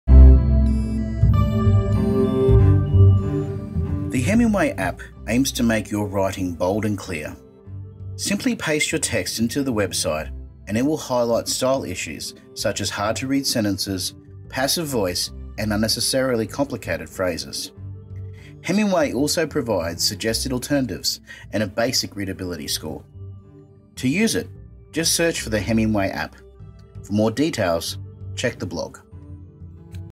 Edvertisement: Hemingway App
The Edvertisement series suggest eLearning Tools that teachers can use for education.

This clip introduces the Hemingway App which helps make your writing bolder and clearer.